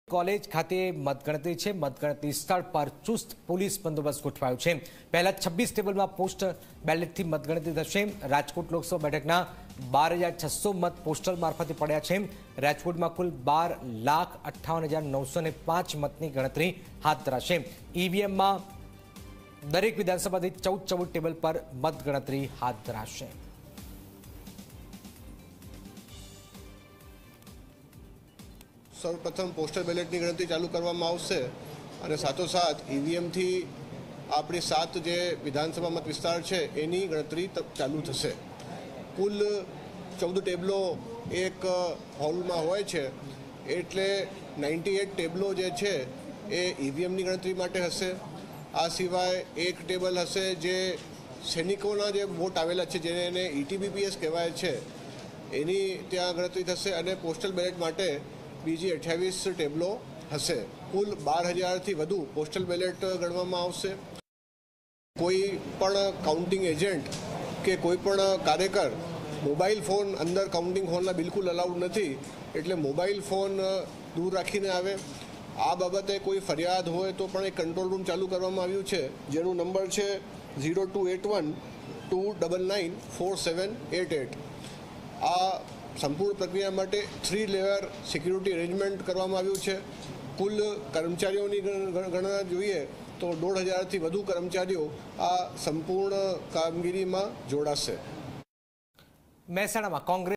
Security beefed up outside counting centre in Rajkot ahead of Lok Sabha election results tomorrow
રાજકોટઃ કણકોટ એન્જિનિયરિંગ કોલેજ ખાતે થશે મતગણતરી---

મતગણતરી સ્થળ પર ચૂસ્ત પોલીસ બંદોબસ્ત ગોઠવાયો---

પહેલાં 26 ટેબલમાં પોસ્ટલ બેલેટથી થશે મત ગણતરી---

રાજકોટ લોકસભા બેઠકનમાં 12,600 મત પોસ્ટલ મારફતે પડ્યા---

રાજકોટમાં કુલ 12 લાખ 58 હજાર 905 મતની ગણતરી હાથ ધરાશે---

EVMમાં દરેક વિધાનસભા દીઠ 14-14 ટેબલ પર થશે મત ગણતરી